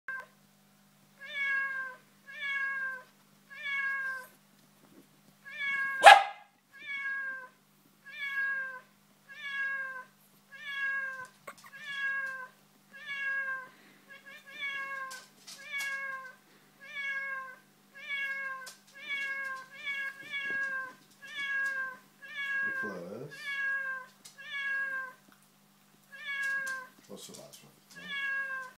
Cute Dog
We were playing with the Cat Piano app on the iPad while the dogs were looking at us all cute and ... View the vid for their reactions.